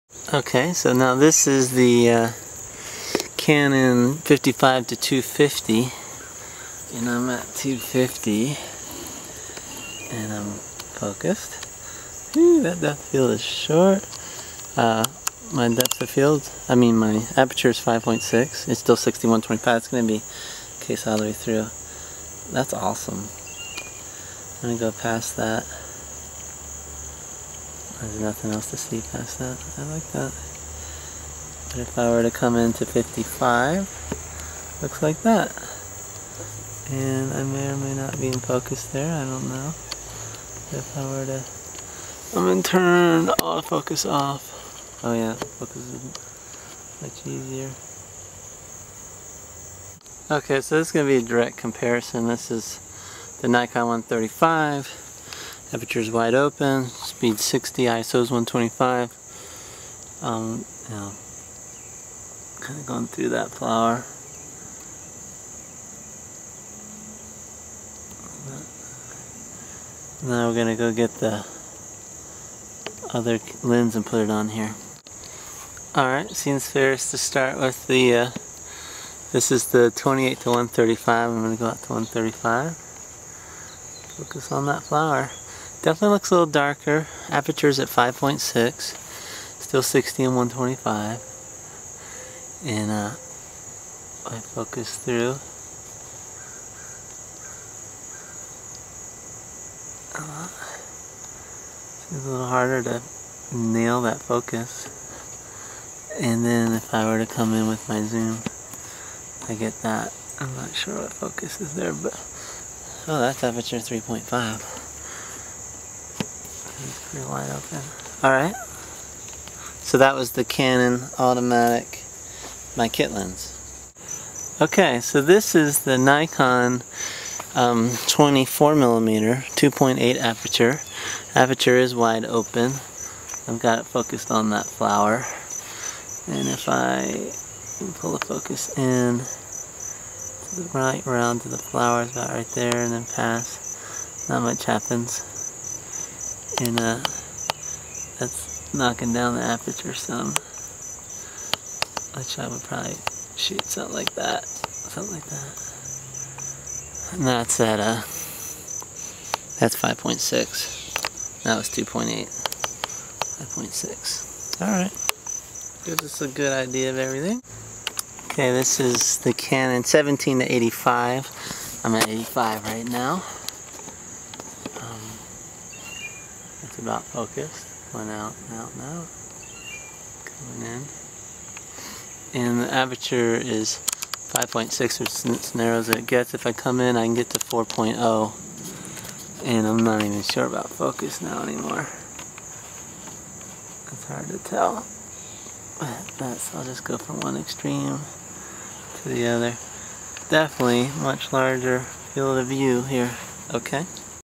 Canon 7D video test, comparing 5 lenses 17mm-250mm
lens specs below, read before you ask please.

Comparing 5 lenses to see difference in focal length, aperture size and depth of field. All on a canon 7D at 720/60fps  iso 125, shutter speed 60, generally speaking the aperture was wide open, except where noted. All shot from same tripod at same spot within minutes of each other. 7:00pm July 27, 2011 Asheville, NC about 2 hours before sunset, right after a rain.

I wasn't planning on sharing this, so I am muttering to myself, but found it useful so I decided to share it. Mostly this is around the recommendation that you get prime lenses to shoot video. I think it depends on if you will be dealing with low light or not. It is hard to focus sometimes with the 7D, especially in low light or if subject is not up in foreground. Audio is from camera.

What do you think of the various bokeh? No surprises the primes let in more light, especially the faster ones. The one surprise was the prime 135 had a bit narrower view angle than the zoom at 135. not sure why that would be.

If all lenses need to be multiplied by 1.6 to get the focal length as the camera reads it, than we are looking at 27mm-400mm of focal length compared.

I used the nikon lenses with a Kawa adapter, I like it.

lenses compared

canon EF-S 55-250 1:4-5.6 IS
canon EF-S 17-85mm 1:4-5.6 IS USM
canon EF-S 28-135mm 1:3.5-5.6 IS USM
nikkor 24mm 1:2.8 prime   798324
nikkor 135mm prime 1:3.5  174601